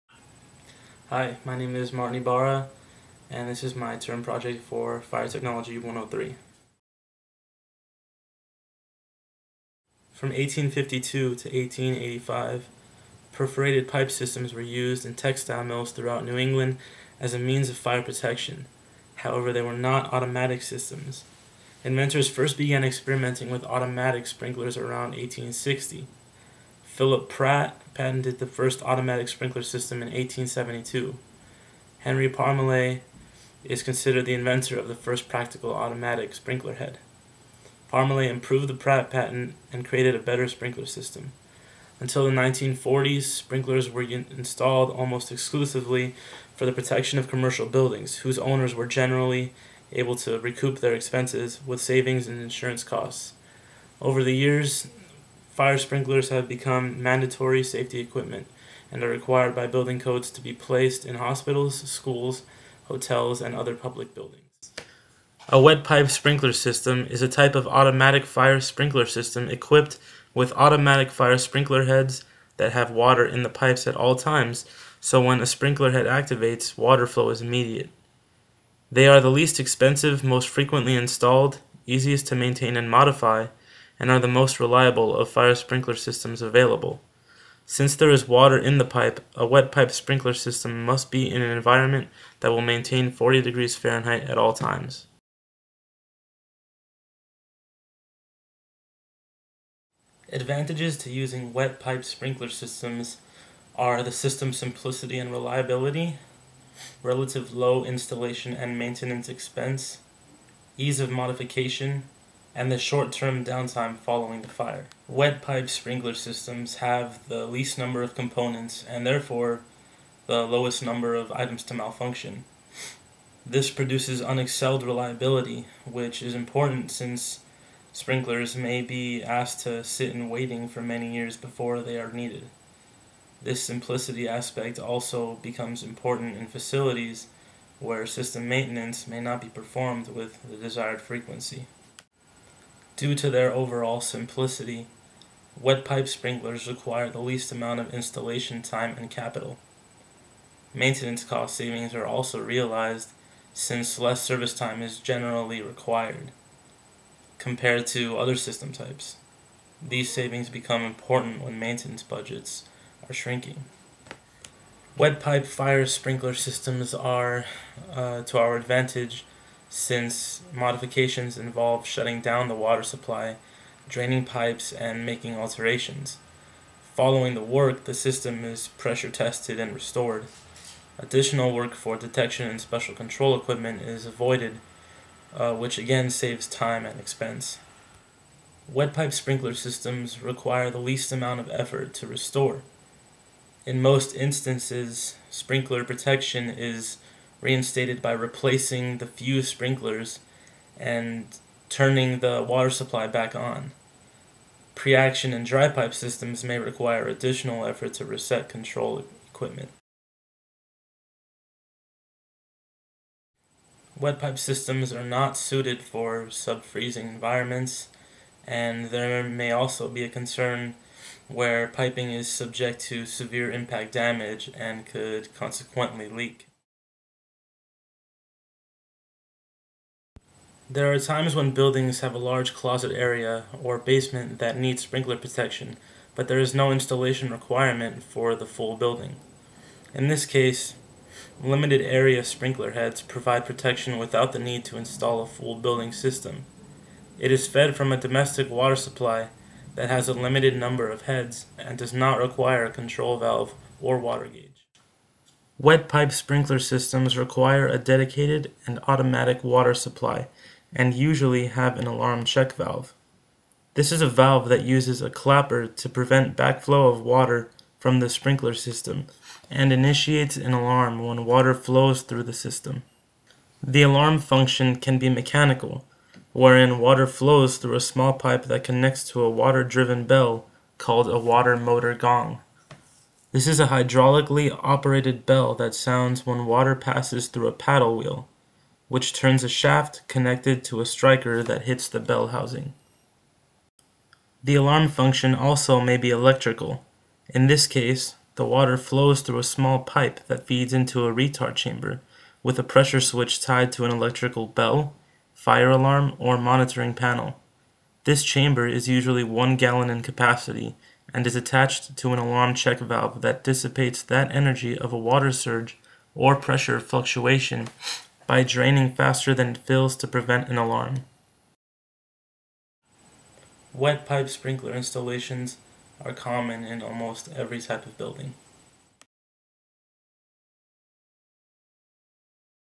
Wet Pipe Sprinkler Systems, Term Project
Explains wet pipe sprinkler systems and its history